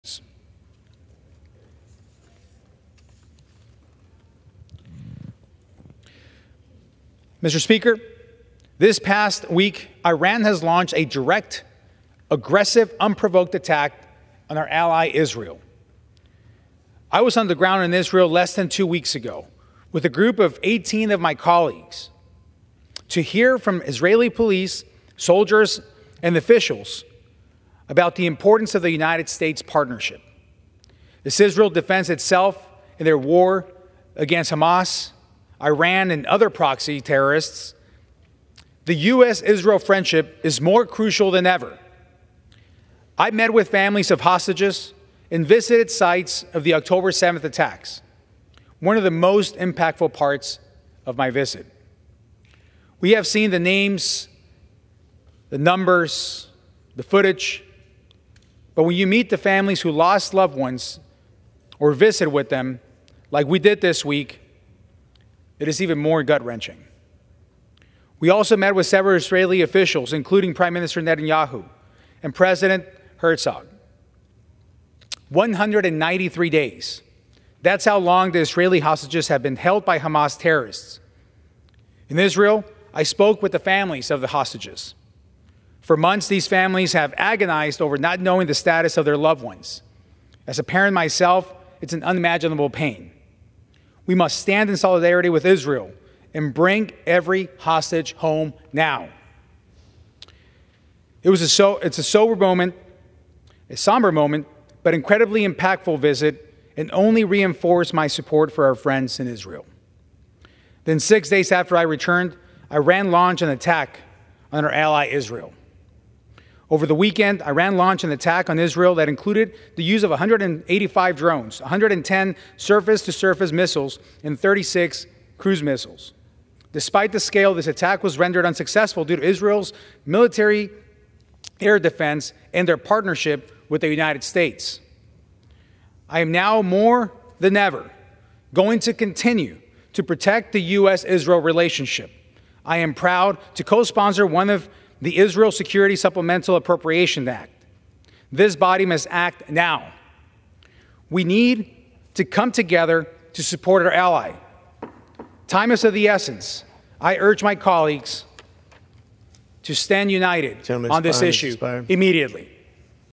Juan Ciscomani Demands US ‘Stand In Solidarity With Israel’ Following Iranian Attacks
During House floor remarks on Wednesday, Rep. Juan Ciscomani (R-AZ) spoke in support of Israel after the Iranian attacks.